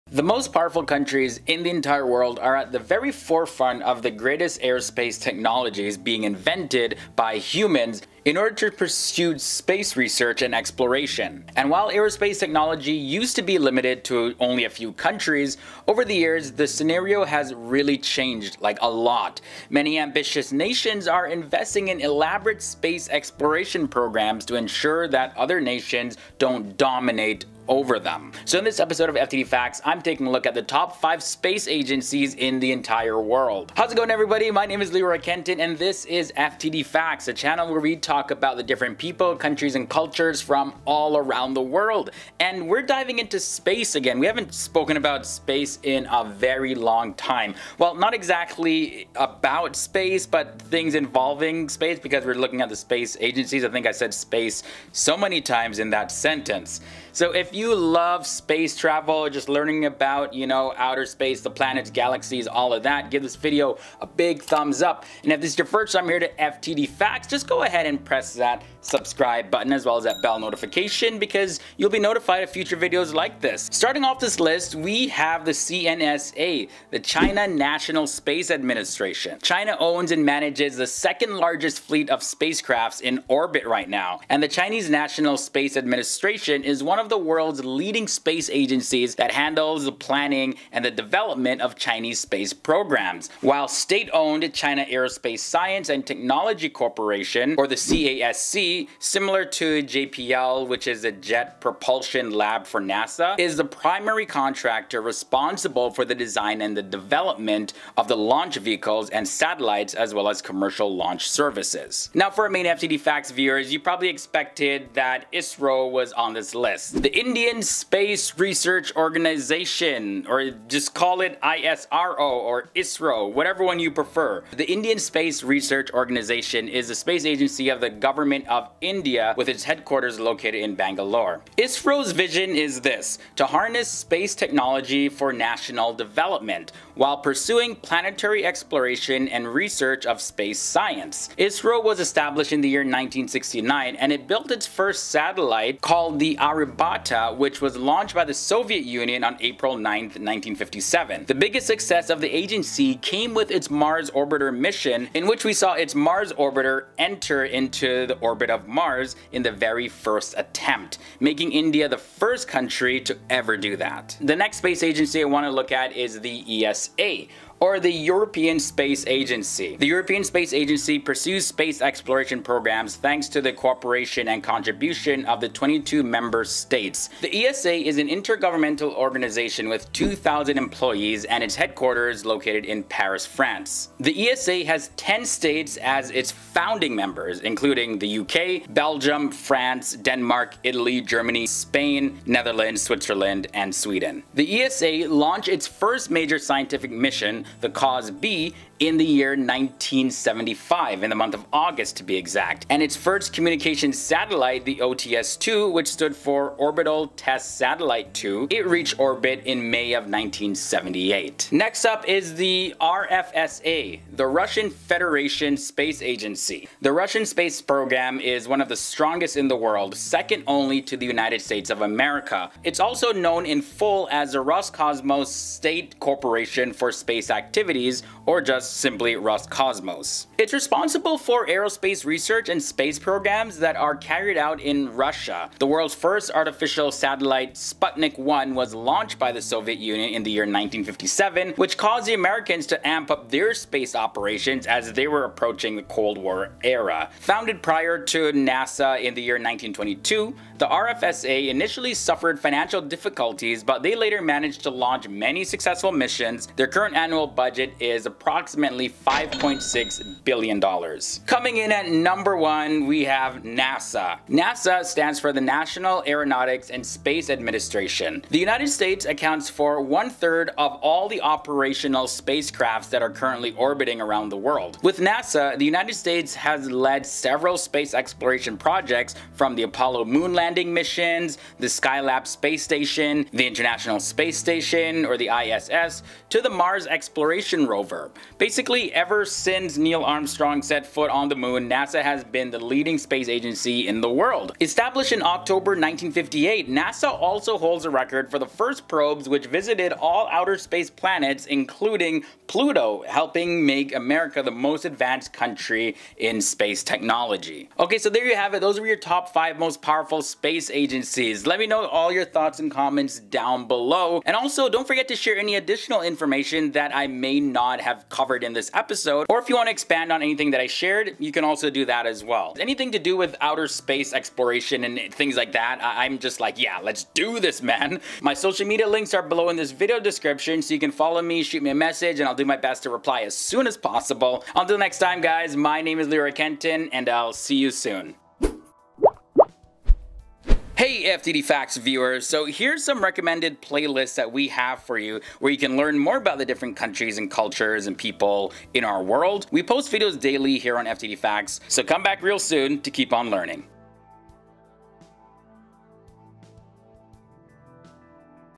The Top 5 Space Agencies In The World
While aerospace technology used to be limited to a few countries, over the years the scenario has changed quite a bit. Many ambitions nations are investing in elaborate space exploration programs to ensure other nations don’t dominate over them. Here are the 5 Most Powerful Space Agencies In The World.